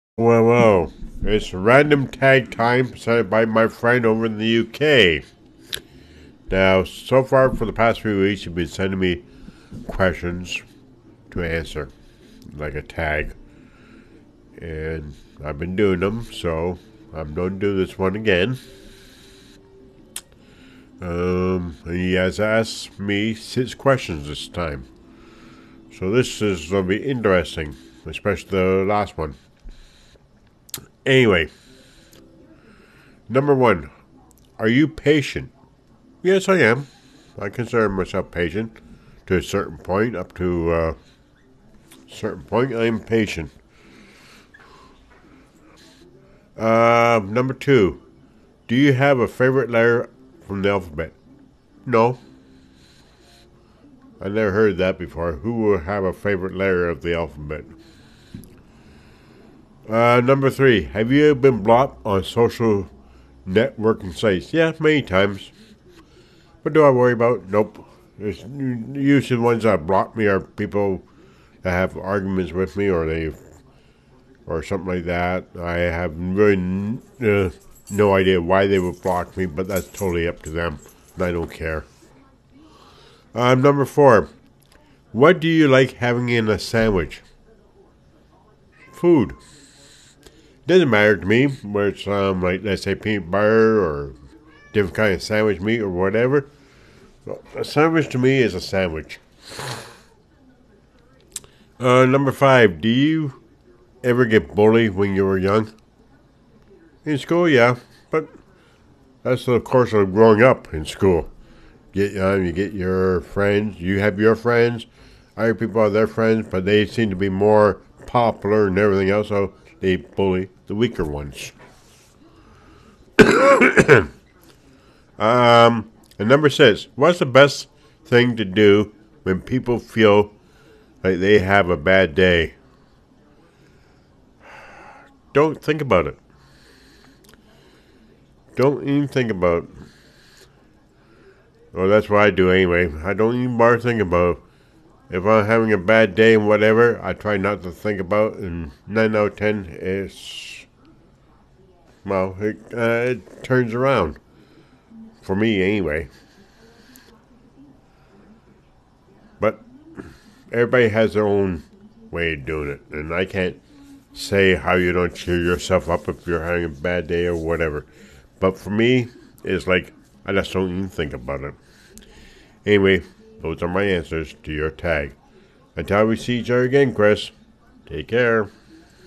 RE: Weekly Random Tag (October 11 2016)
QUESTIONS:
1) Are you patient?
2) Do you have a favourite letter from the alphabet?
3) Have you ever been blocked on social networking sites?
4) What do you like having in a sandwich?
5) Did you ever get bullied when you were young?
6) What is the best thing to do when people feel like they have had a bad day?